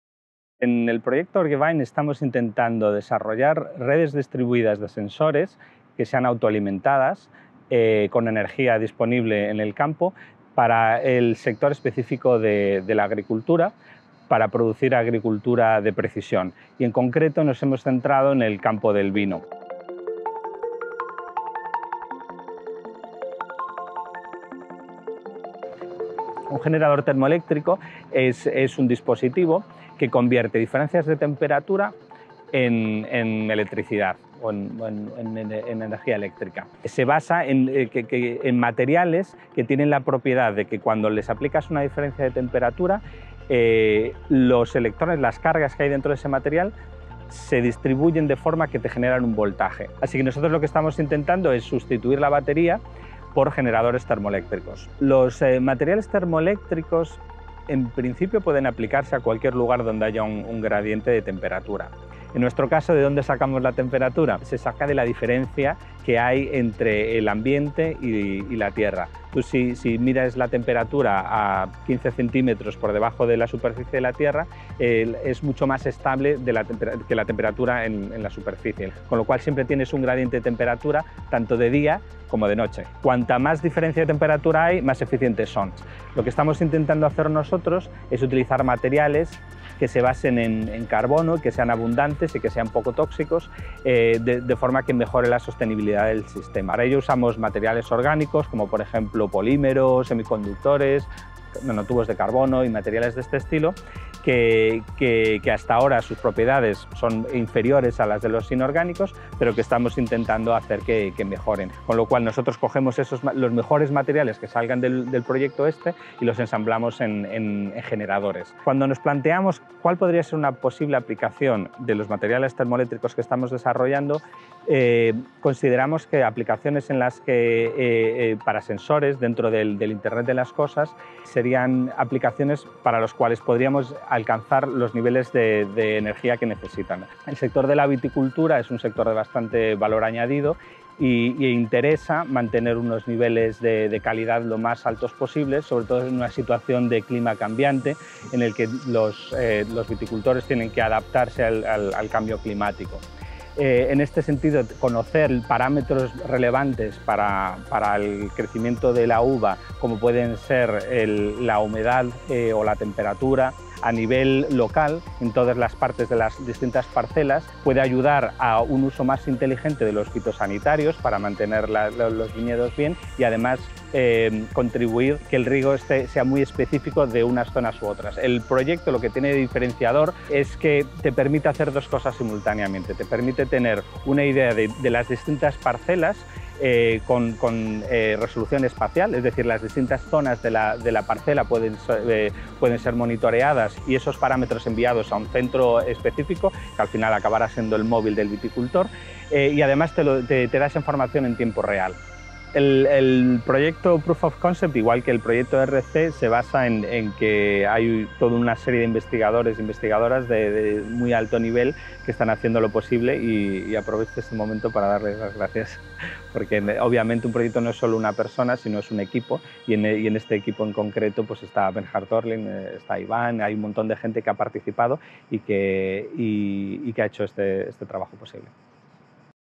Organic thermoelectric generators for precision viticulture sensors - Mariano Campoy-Quiles | ICMAB
On July 2020, Mariano Campoy-Quiles, researcher at the Nanostructured Materials for Optoelectronics and Energy Harvesting (NANOPTO) group, was granted with an ERC Proof of Concept (PoC) to advance in the development of organic thermoelectric generators to power low-cost sensors for vineyards.

The awarded project, ORGEVINE “ORganic thermoelectric GEnerators to power precision VIticulture sensor Networks” aims to reduce water and phytosanitary chemicals usage as well as enhancing production yield in the wine industry by developing a network of wireless autonomous sensors that will provide real time images of key field parameters such as temperature and humidity.